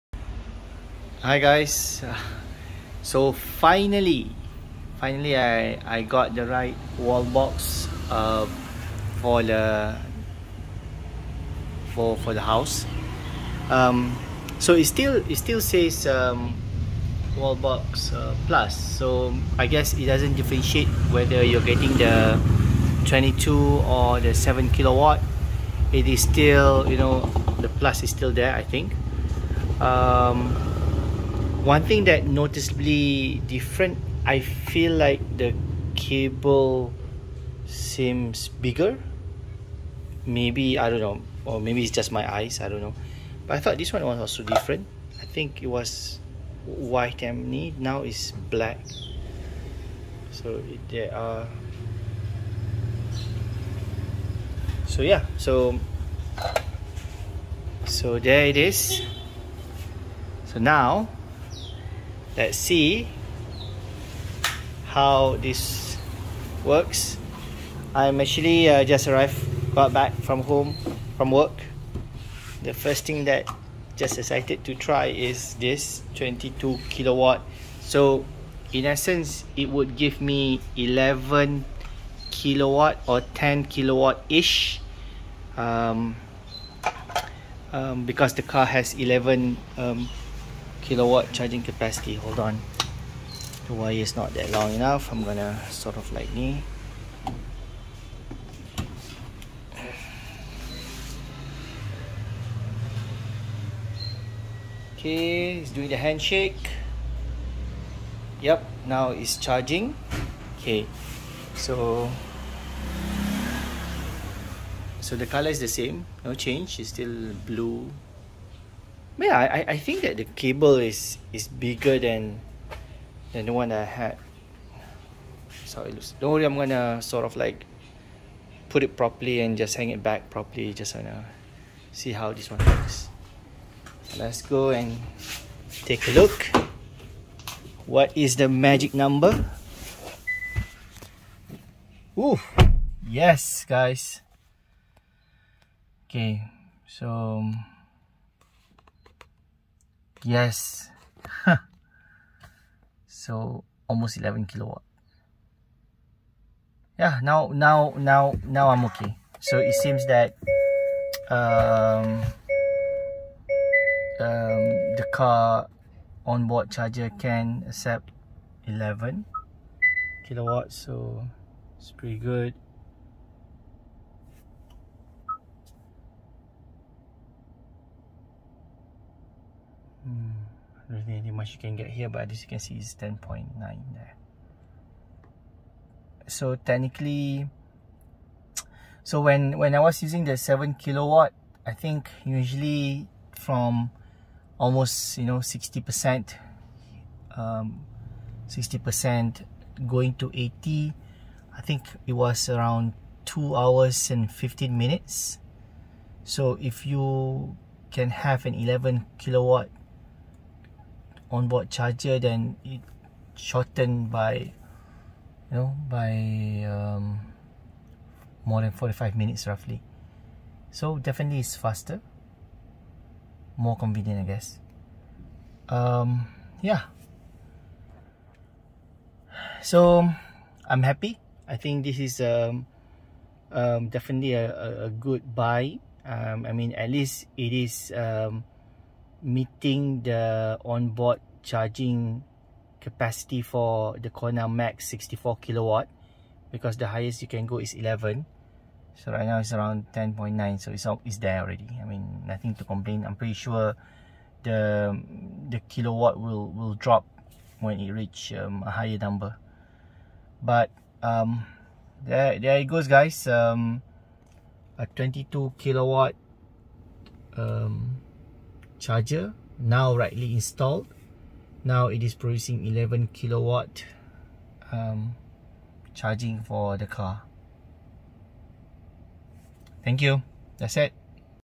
Mystery solved! Finally charging 11kW into Kona Electric (64kW)
It has been a perplexed one week because I could only get around 7.1kW into the car. Apparently I was provided a smaller capacity Wallbox!